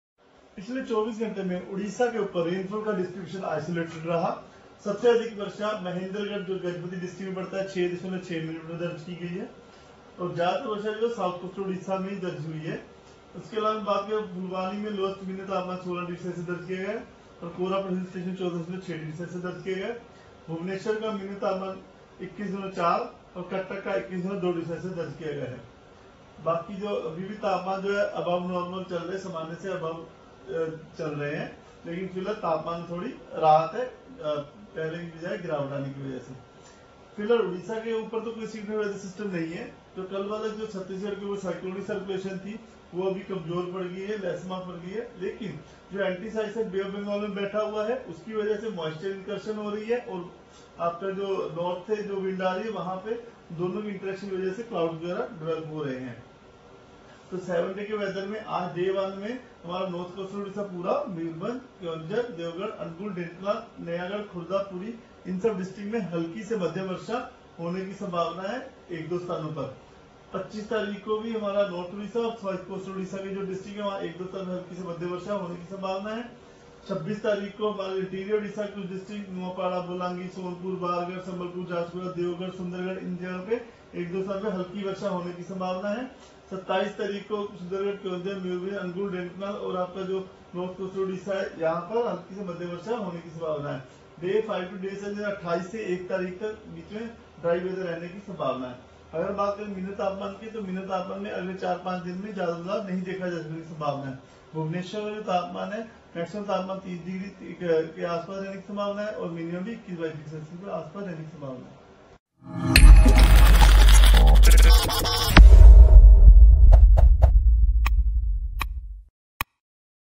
24 February Weather Report । କେତେଦିନ ଲାଗିରହିବ କୋହଲା ପାଗ ସାଙ୍ଗକୁ ବର୍ଷା, କଣ କହୁଛି ପାଣିପାଗ ବିଭାଗ
ଦ୍ବୈତ ନଗରୀ କଟକ ଓ ଭୁବନେଶ୍ବର ସମେତ ରାଜ୍ୟର ବିଭିନ୍ନ ସ୍ଥାନରେ ଆଜି ସକାଳୁ କୋହଲା ପାଗ ପରିଲକ୍ଷିତ ହୋଇଛି। ସକାଳେ ରାଜଧାନୀ ଭୁବନେଶ୍ବରରେ ମଧ୍ୟ ହାଲୁକା ବର୍ଷା ହୋଇଛି। ଏତଦବ୍ୟତୀତ ଉପକୂଳ ଓଡ଼ିଶାର ଭଦ୍ରକ, କେନ୍ଦ୍ରାପଡ଼ା, କଟକ, ନୟାଗଡ଼, ଖୋର୍ଦ୍ଧା, ପୁରୀ, ତଥା ଆଭ୍ୟନ୍ତରୀଣ ଓଡ଼ିଶାର ଅନୁଗୁଳ, କେନ୍ଦୁଝର, ଢେଙ୍କାନାଳର କିଛି ଜିଲ୍ଲାରେ ମେଘୁଆ ଆକାଶ ସହ କୋହଲା ପାଗ ଦେଖିବାକୁ ମିଳିଛି। ଏହାଛଡ଼ା  ଉପକୂଳ ଓଡ଼ିଶାର କିଛି ଜିଲ୍ଲାରେ ହାଲୁକା ବର୍ଷା ବି ହୋଇଥିବାର ଖବର ମିଳିଛି।

About Channel:-